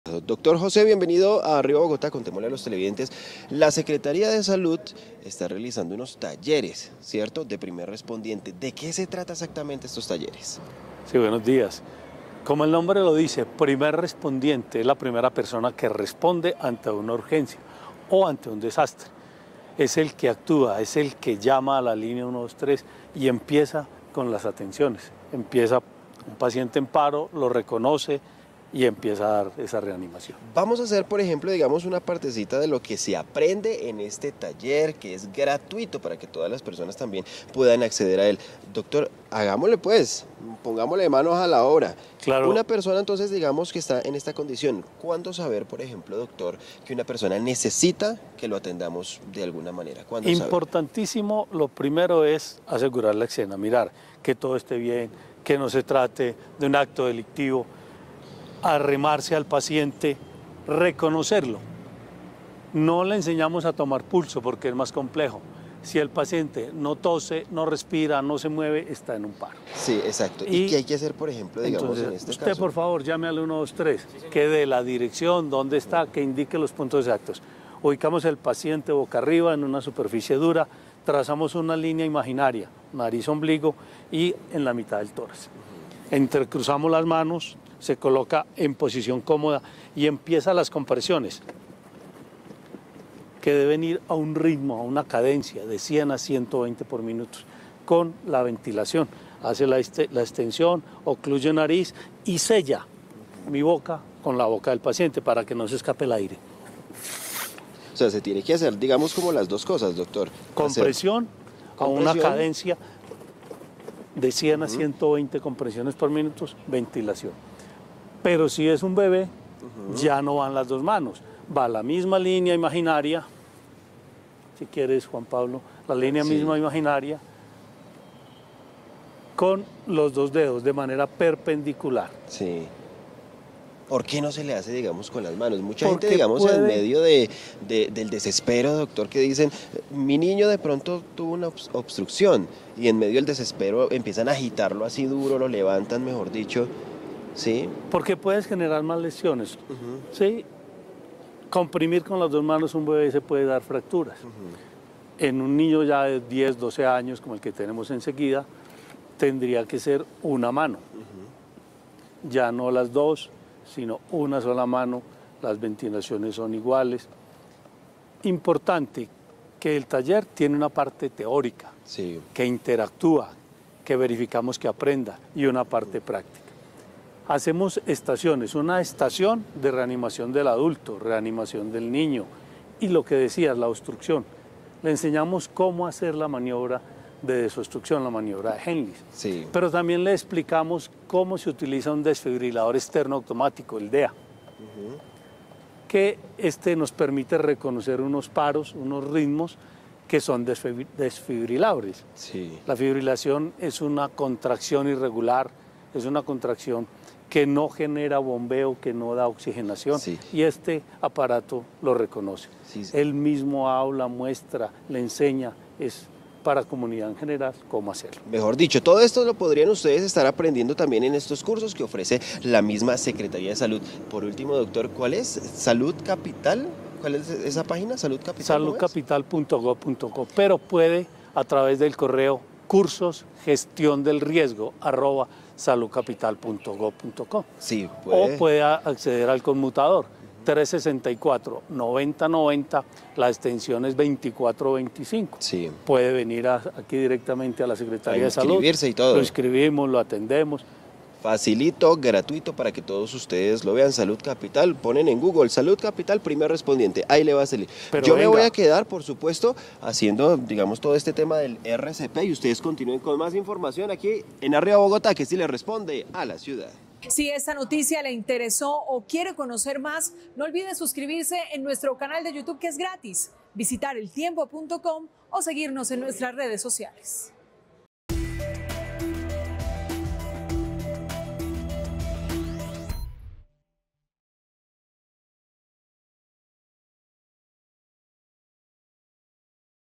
Taller gratuito para primeros auxilios | El Tiempo
Rajados en primeros auxilios. De acuerdo con cifras de la Secretaría de Salud, menos del 10% de los bogotanos saben practicar una reanimación cardio pulmonar o brindarle ayuda a una persona que esté en riesgo por alguna emergencia. Por esta razón, la entidad retomó los cursos gratuitos de primer respondiente.